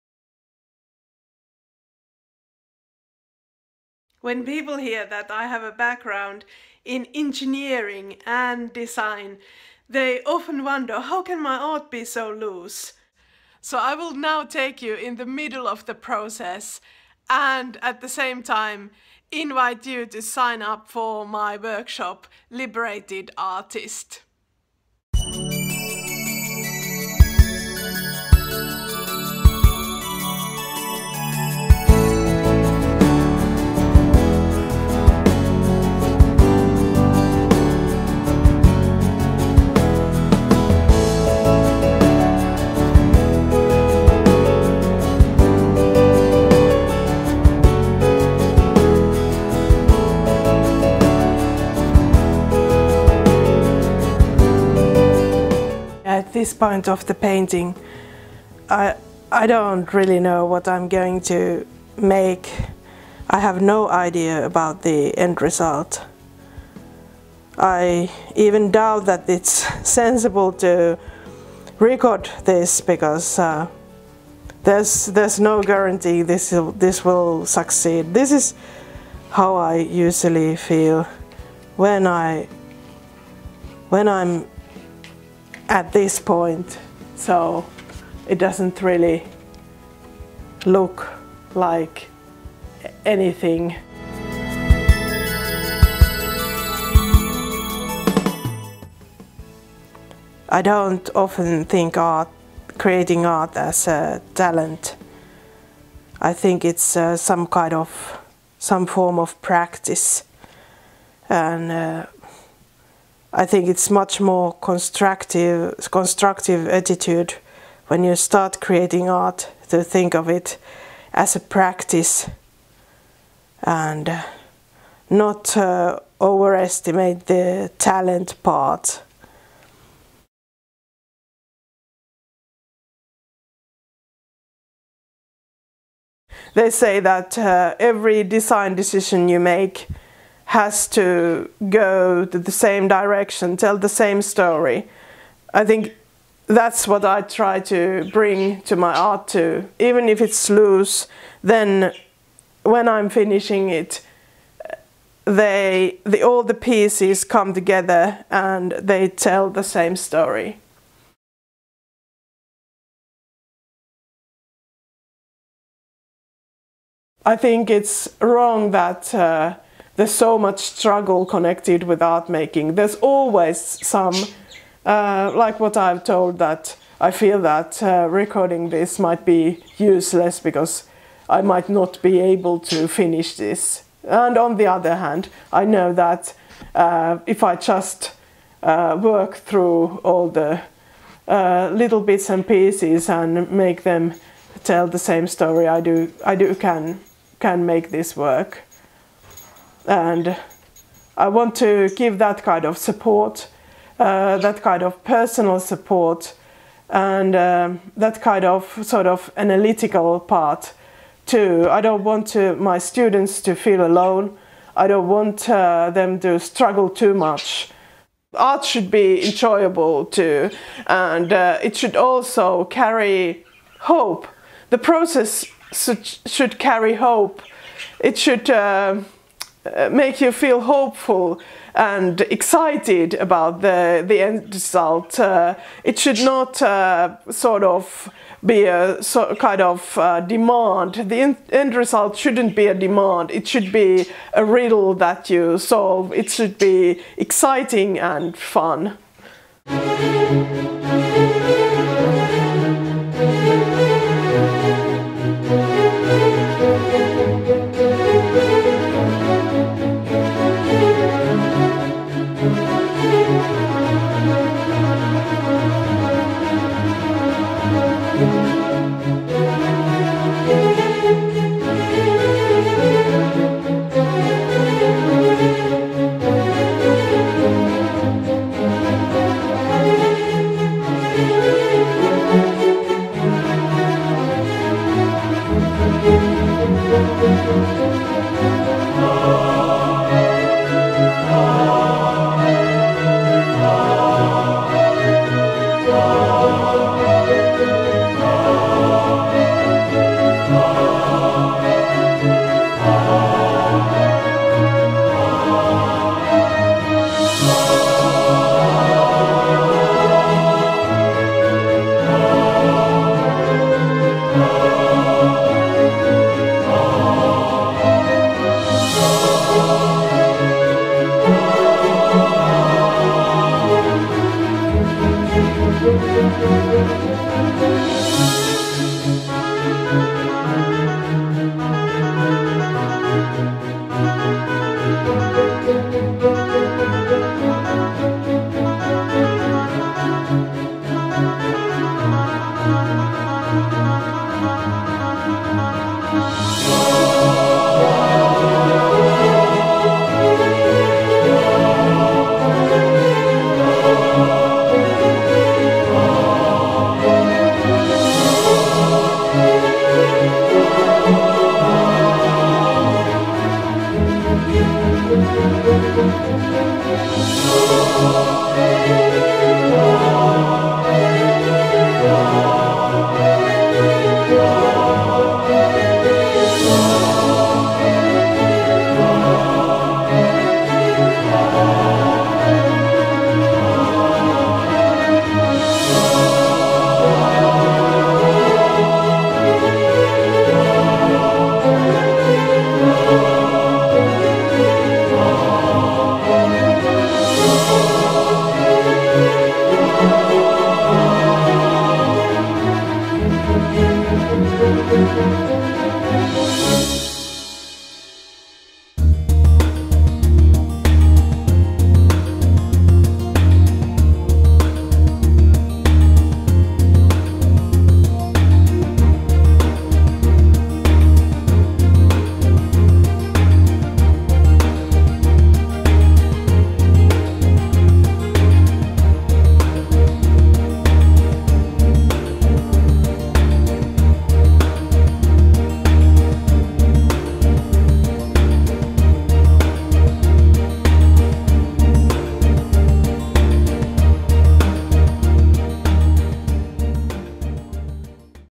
Painting in Liberated Style
See how a colorful mess becomes a focused painting. Hear my thoughts about creating and teaching art.